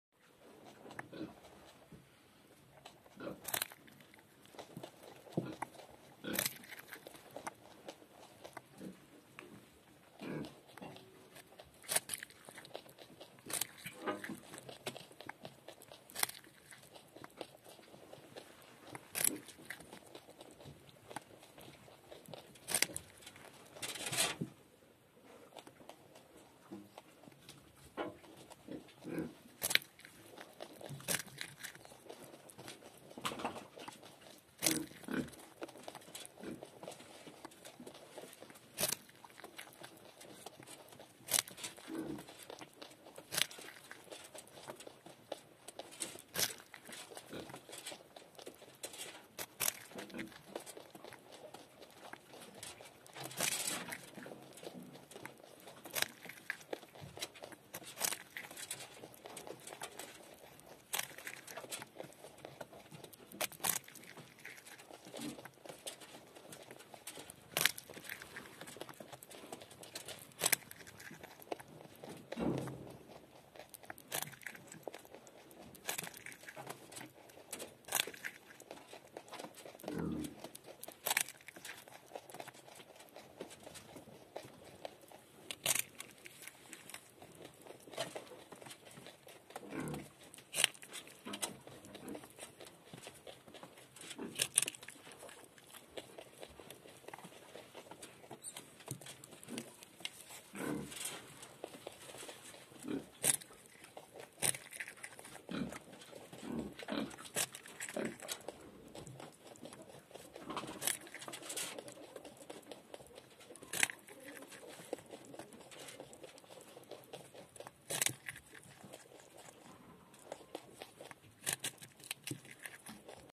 Нутрия забавно кушает морковку
Нутрия очень забавно кушает морковку. За ними всегда очень интересно наблюдать а особенно когда они что то грызут.